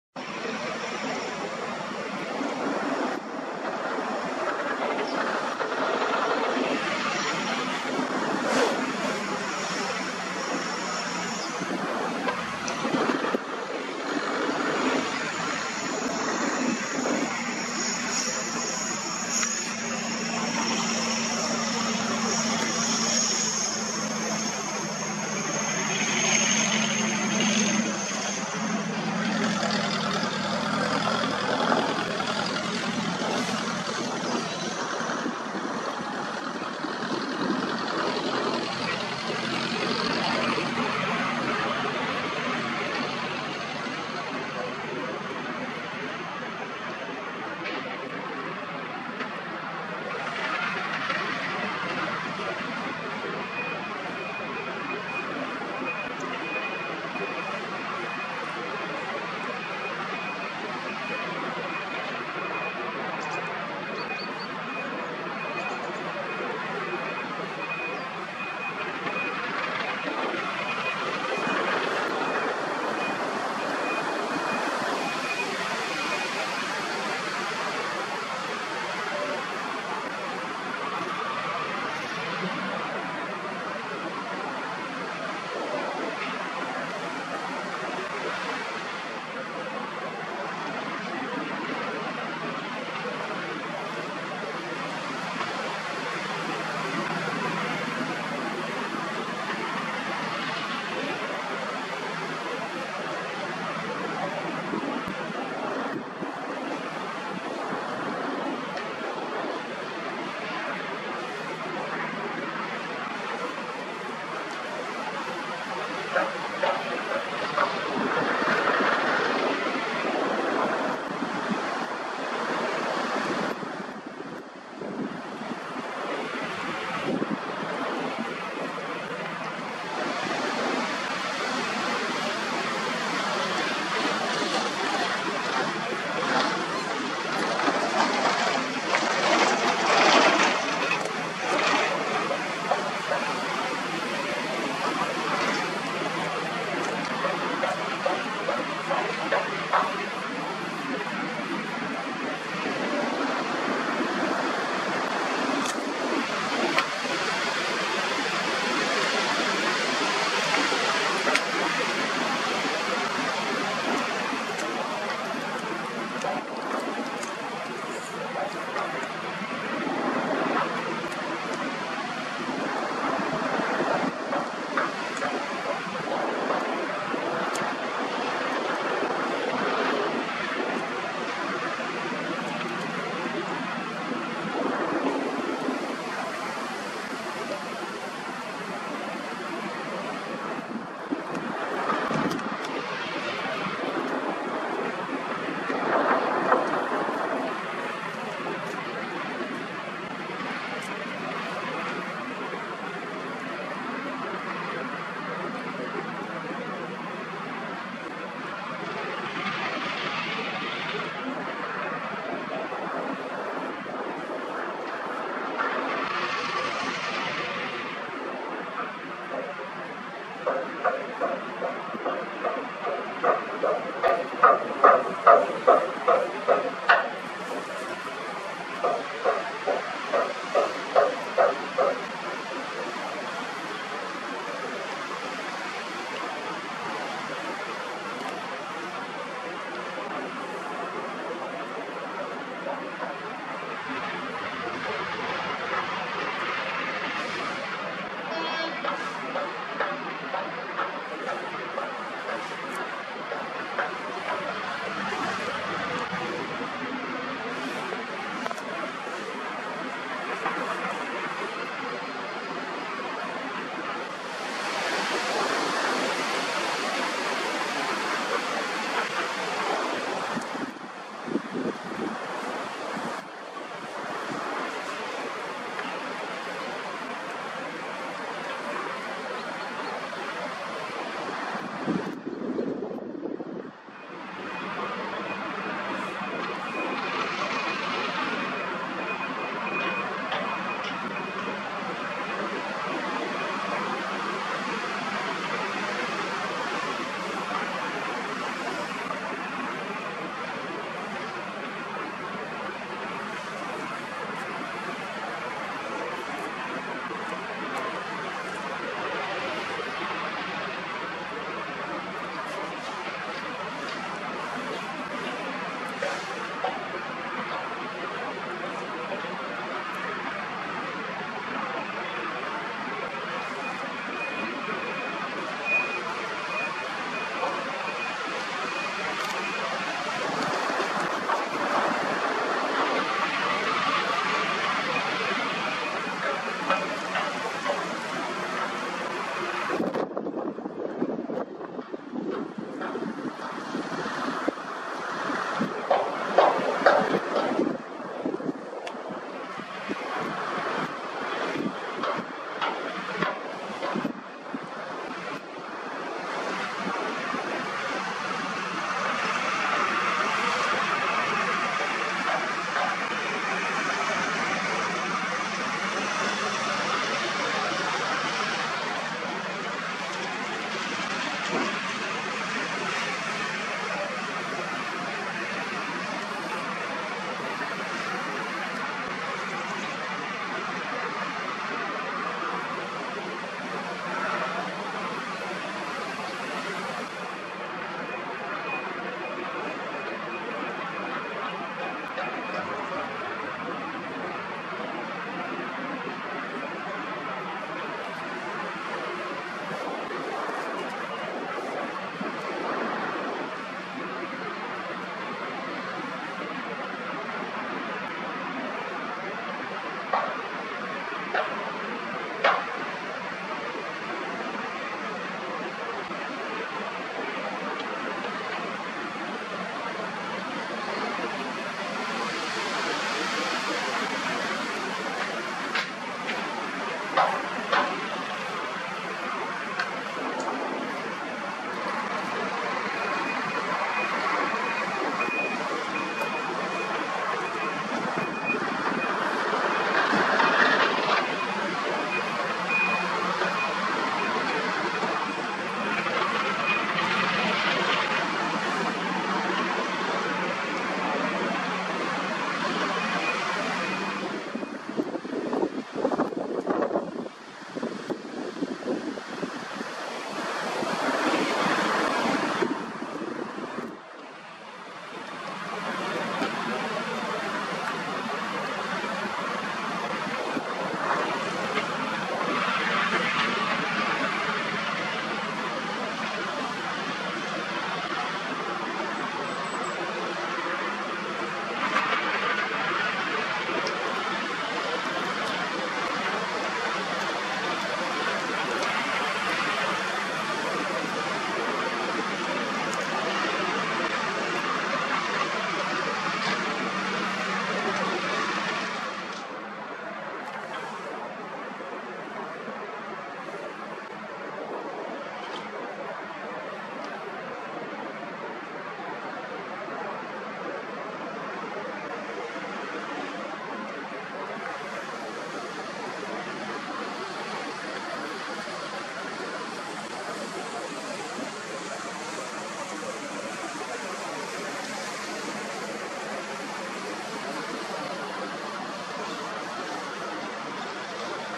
Przekop Mierzei Wiślanej -awaria. Wyciek na łączeniu rury refulacyjnej z pogłębiarką Trojan.
Przekop Mierzei Wiślanej awaria. Wyciek na łączeniu rury refulacyjnej z pogłębiarką Trojan. Budowlańcy starają się rozwiązać problem.
Film wykonany Nikonem P1000  z 125-krotnym zoomem. Nagranie z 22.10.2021.